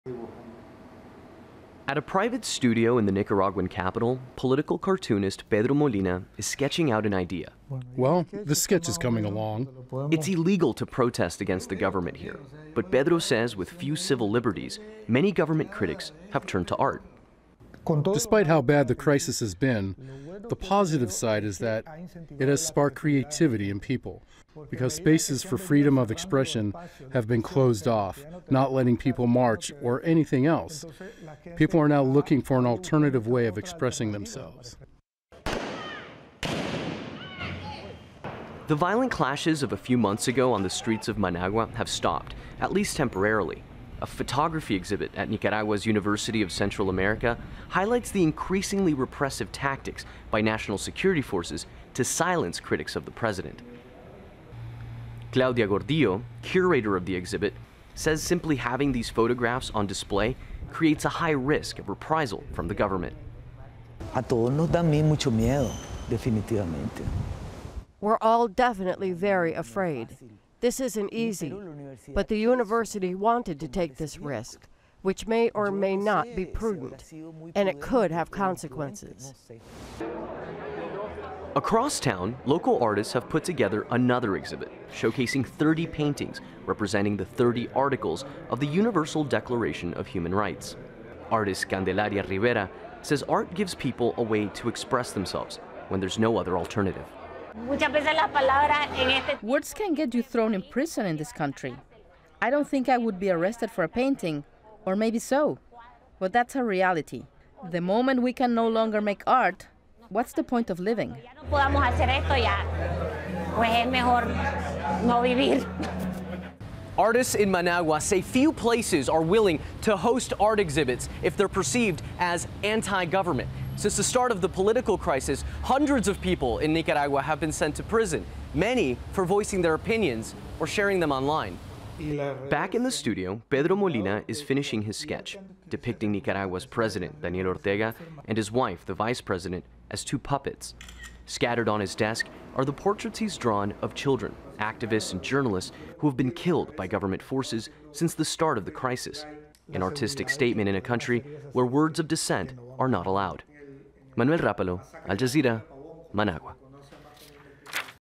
🇳🇮Nicaraguan cartoonist resists government crackdown through art | Al Jazeera English
A deepening political crisis in Nicaragua has prompted President Daniel Ortega to crack down on his critics.
He has expelled international monitors who had been documenting his government's alleged crimes that have killed more than 300 protesters since April. That's pushing Nicaraguans to find alternative forms of protest - such as art.

Al Jazeera's Manuel Rapalo meets one such artist from the capital, Managua, who has since had his studio raided by members of the national police.